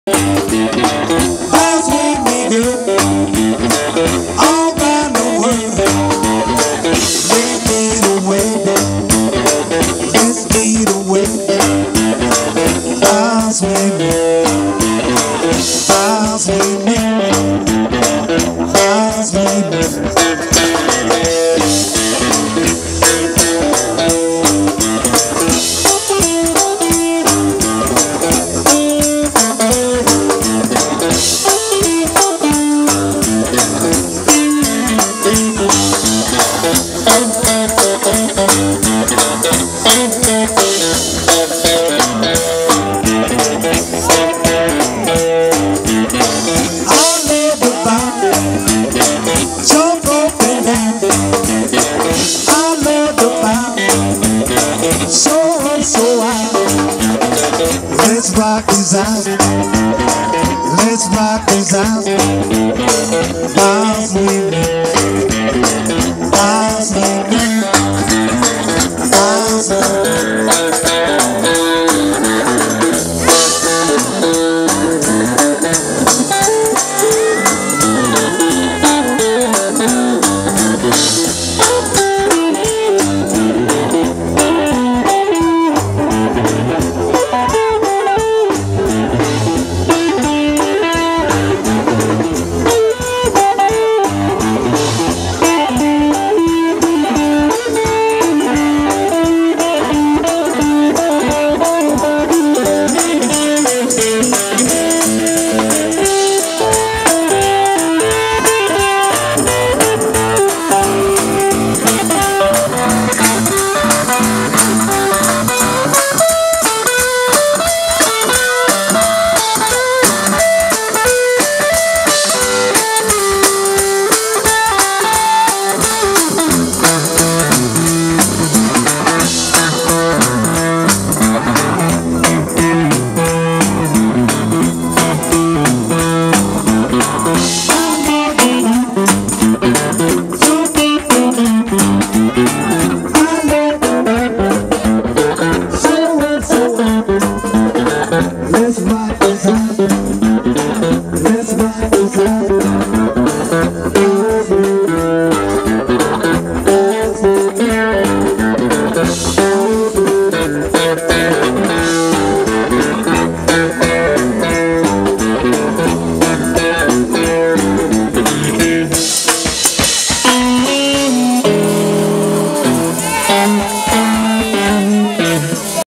091 Lightnin Malcolm and Kinney Kimbrough Live at R L Boyce Picnic 2018
Hill Country veteran Lightnin Malcolm with the legendary Kinney Kimbrough on drums at the 2nd Annual R. L. Boyce Picnic in Como, Mississippi, 8/17/18